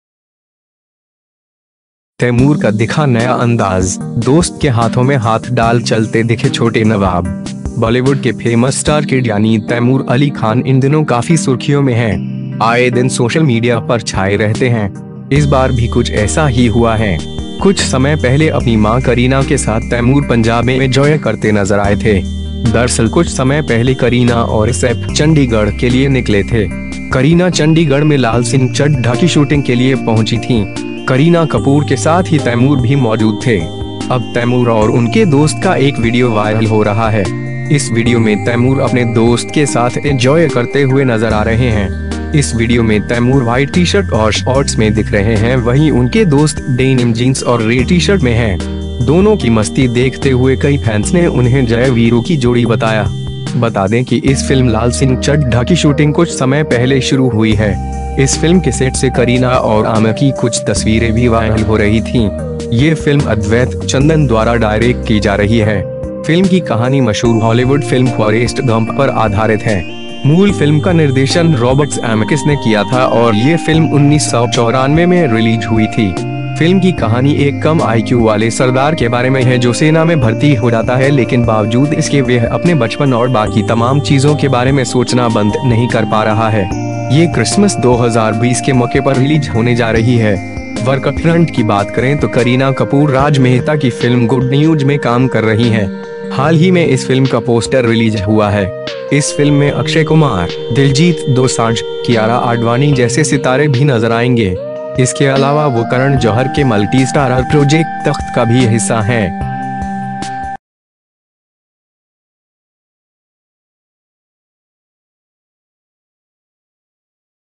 तैमूर का दिखा नया अंदाज, दोस्त के हाथों में हाथ डाल चलते दिखे 'छोटे नवाब
बॉलीवुड के फेमस स्टार किड यानी तैमूर अली खान इनदिनों काफी सुर्खियों में हैं। आए दिन सोशल मीडिया पर छाए रहते हैं। इस बार भी कुछ ऐसा ही हुआ हैं। कुछ समय पहले अपनी मां करीना के साथ तैमूर पंजाब में एंजॉय करते नजर आए थे। दरअसल कुछ समय पहले करीना और सैफ चंडीगढ़ के लिए निकले थे। करीना चंडीगढ़ में लाल सिंह चड्ढा की शूटिंग के लिए पहुंची थीं।

करीना कपूर के साथ ही तैमूर भी मौजूद थे। अब तैमूर और उनके दोस्त का एक वीडियो वायरल हो रहा है। इस वीडियो में तैमूर अपने दोस्त के साथ एंजॉय करते हुए नजर आ रहे हैं। इस वीडियो में तैमूर व्हाइट टी शर्ट और शॉर्ट्स में दिख रहे हैं वही उनके दोस्त डेनिम जींस और रेड टी-शर्ट में हैं। दोनों की मस्ती देखते हुए कई फैंस ने उन्हें जय-वीरु की जोड़ी बताया।

बता दें कि इस फिल्म लाल सिंह चड्ढा की शूटिंग कुछ समय पहले शुरु हुई है। इस फिल्म के सेट से करीना और आमिर की कुछ तस्वीरें भी वायरल हो रही थीं। ये फिल्म अद्वैत चंदन द्वारा डायरेक्ट की जा रही है। फिल्म की कहानी मशहूर हॉलीवुड फिल्म फॉरेस्ट गम्प पर आधारित है।

मूल फिल्म का निर्देशन Robert Zemeckis ने किया था और ये फिल्म 1994 में रिलीज हुई थी। फिल्म की कहानी एक कम आईक्यू वाले सरदार के बारे में है जो सेना में भर्ती हो जाता है लेकिन बावजूद इसके वह अपने बचपन और बाकी तमाम चीजों के बारे में सोचना बंद नहीं कर पा रहा है।

ये क्रिसमस 2020 के मौके पर रिलीज होने जा रही है। वर्कफ्रंट की बात करें तो करीना कपूर राज मेहता की फिल्म गुड न्यूज में काम कर रही हैं।

हाल ही में इस फिल्म का पोस्टर रिलीज हुआ है। इस फिल्म में अक्षय कुमार, दिलजीत दोसांझ, कियारा आडवाणी जैसे सितारे भी नजर आएंगे। इसके अलावा वो करण जौहर के मल्टीस्टारर प्रोजेक्ट तख्त का भी हिस्सा हैं।